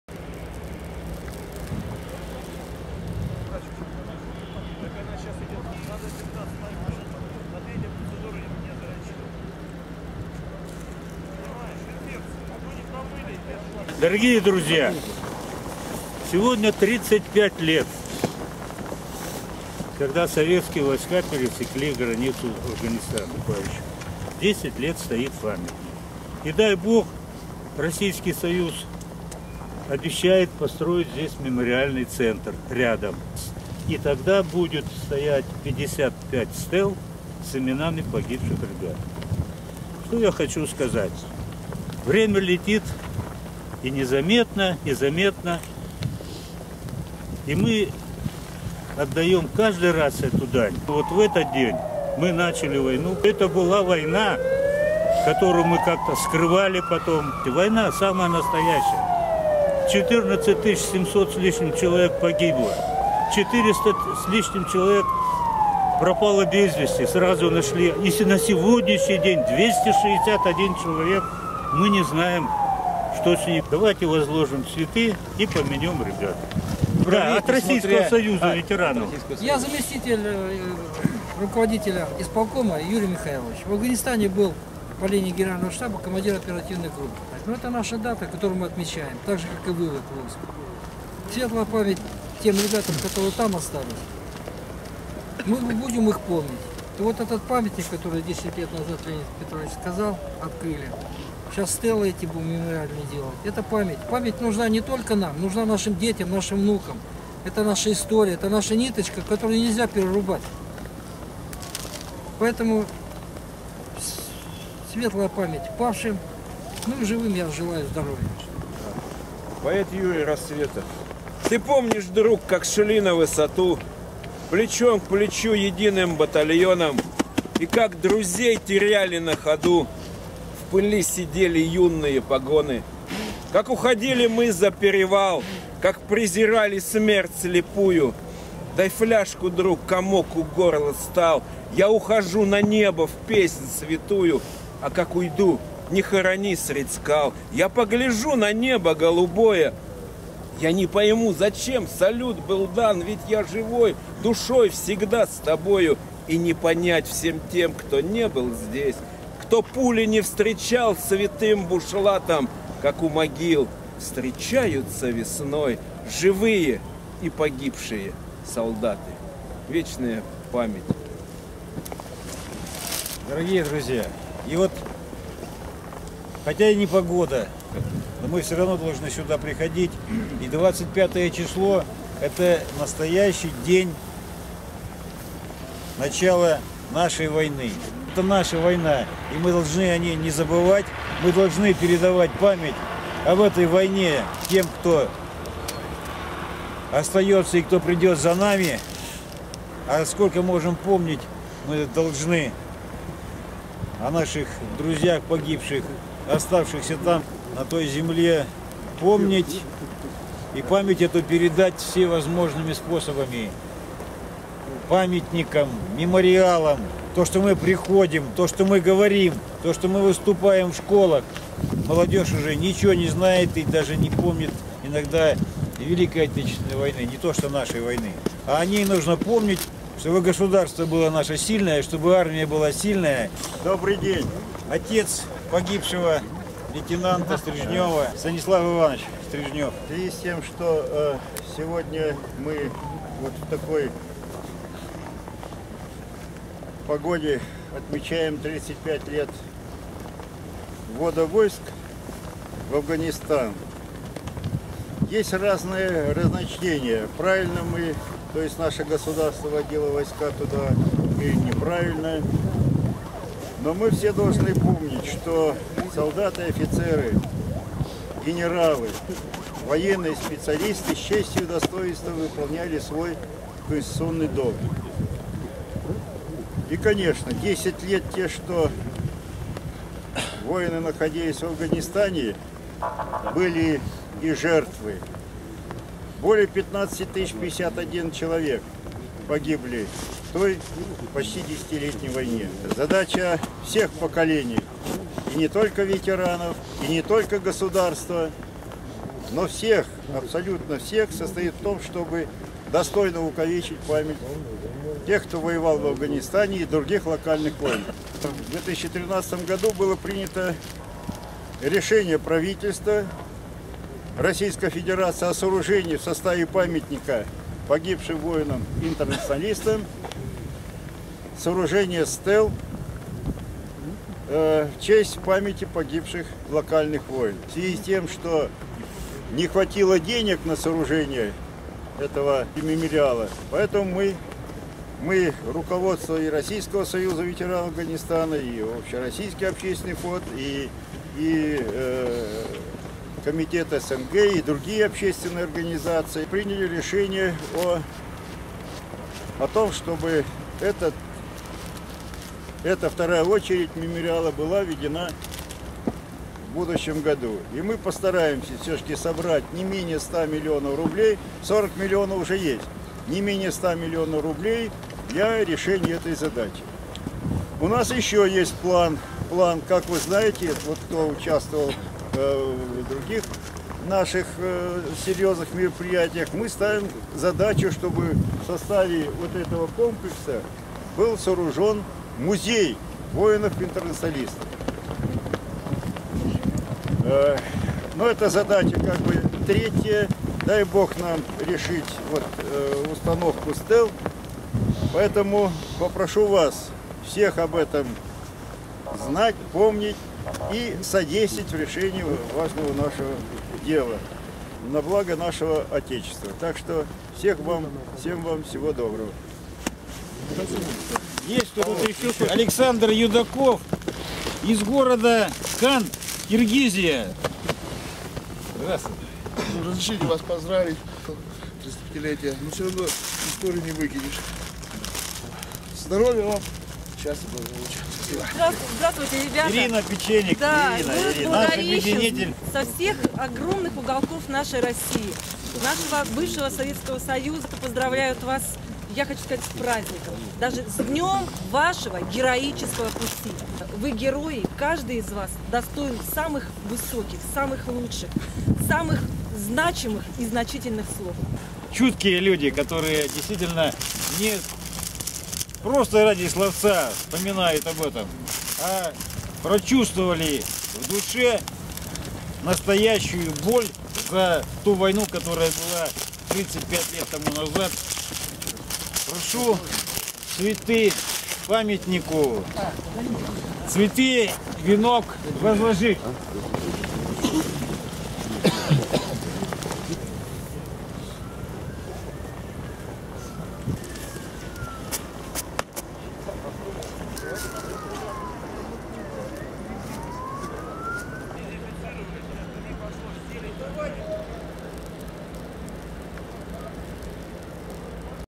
35-летие Афганской войны на Поклонной горе
Участники войны в Афганистане, родственники и друзья погибших воинов, Фонд инвалидов войны в Афганистане, представители других общественных организаций 25 декабря в день начала войны в Афганистане почтили память погибших за интересы Родины у памятника Воину-интернационалисту на Поклонной горе в Москве.
Ярославская региональная общественная организация ветеранов войны в Афганистане призывает всех неравнодушных соотечественников поддержать инициативу создания мемориала памяти всех воинов павших за интересы России!!! Советская 19.